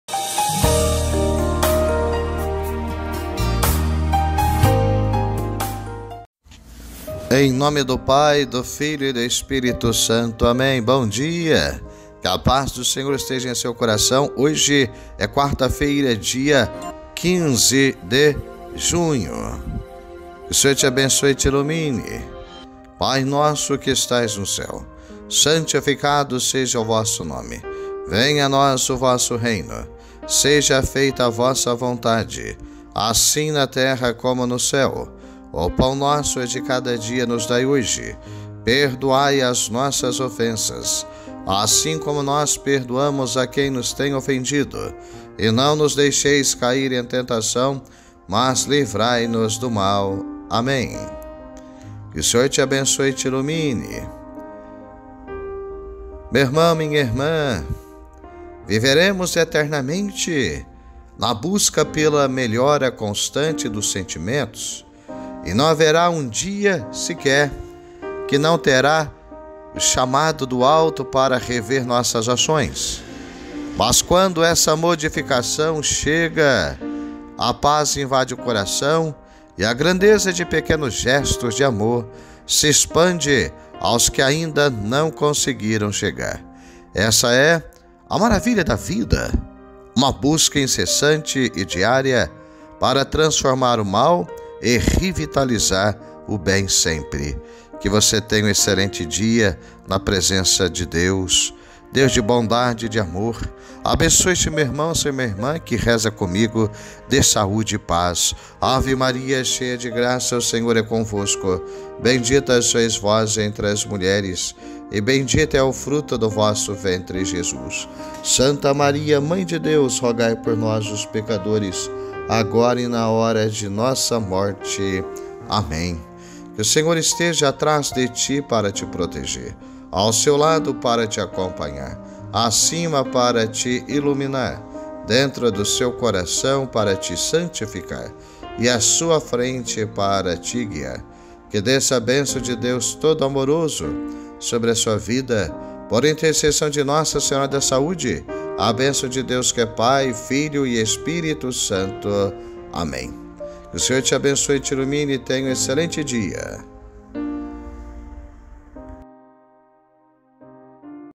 ORAÇÃO DA MANHÃ com Padre Donizetti de Brito.
15/06/2022 - Comece o dia voltando o seu coração para Deus com a Oração da Manhã.